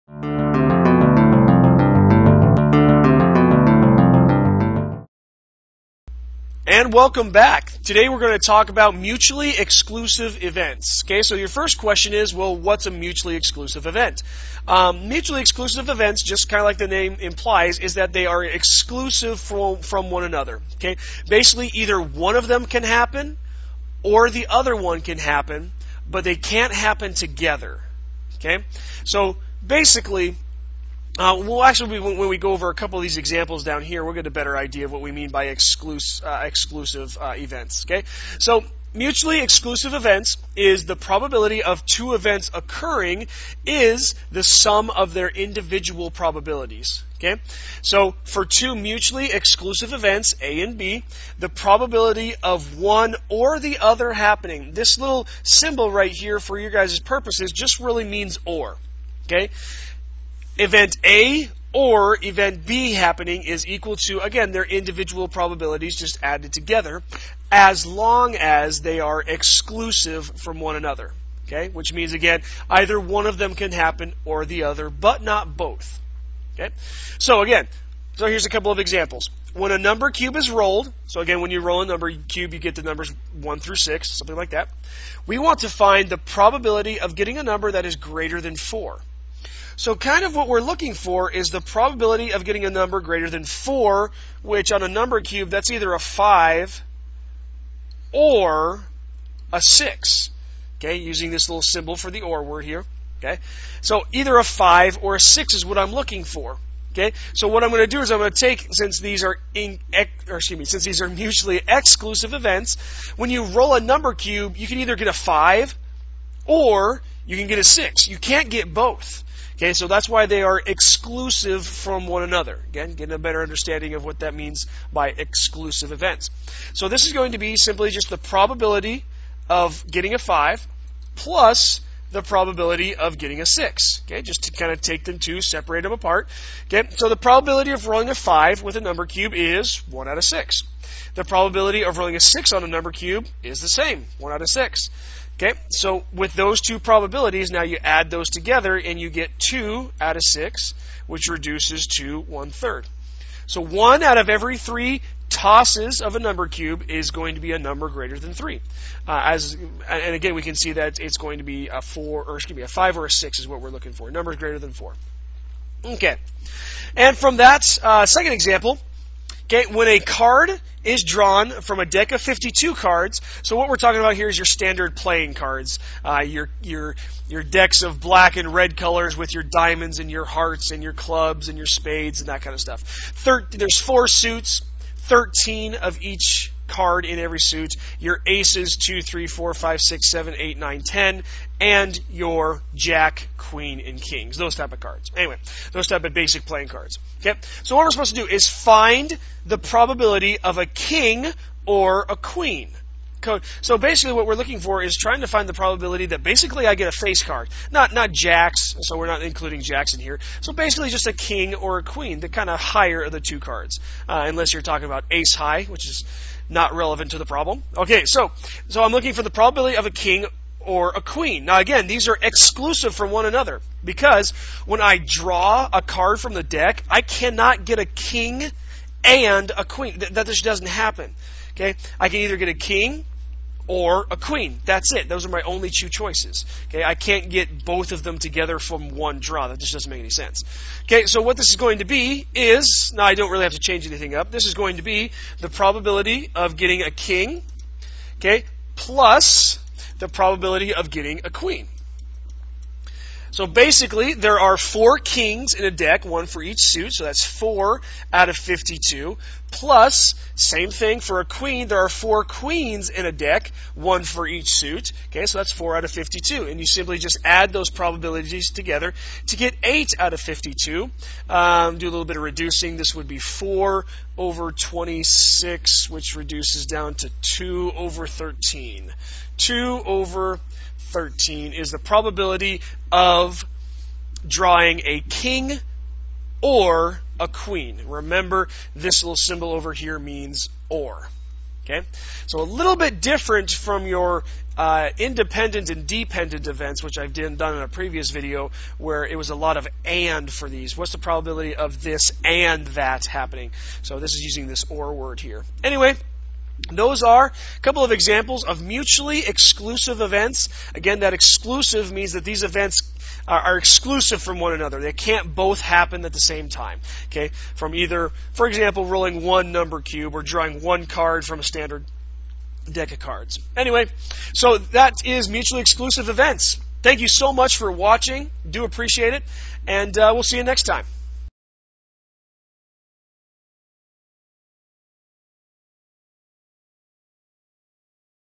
Mutually Exclusive Events
A short video on how to find the probability of Mutually Exclusive Events.